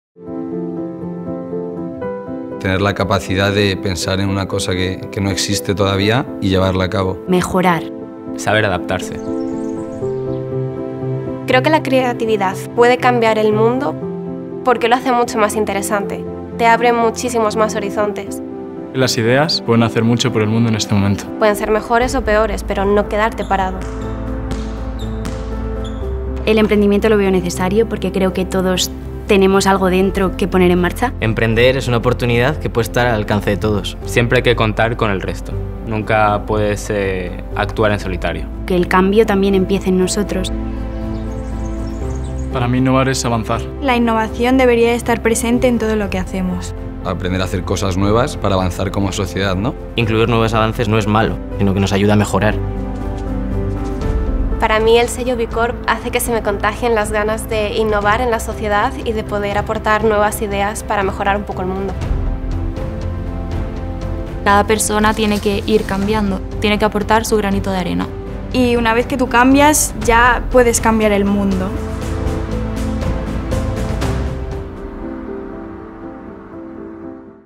Y tú, ¿qué quieres cambiar? | UCJC
El mundo está en constante cambio, y tú puedes ser quien lo impulse. En la Universidad Camilo José Cela te ayudamos a formarte para liderar el cambio del futuro. Descubre todo lo que podemos ofrecerte.